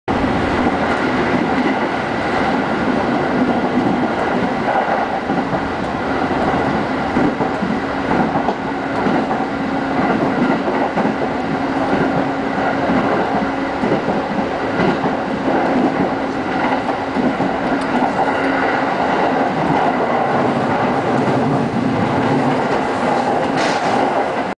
湘南新宿ラインの風景2006 鶴見駅付近 寝台特急出雲１号を追い抜く 2006． 1. 9
説明   湘南新宿ライン2300Y快速国府津行きが並走する寝台特急「出雲１号」を追い抜く。　動画の冒頭で通過した駅は「鶴見駅」。停車の電車はもちろん京浜東北線の209系だ。踏切道の照明が一瞬、出雲１号の先頭に立つ牽引機「EF65-1000」にスポットライトを当てる。　京浜急行の電車ともすれちがっている。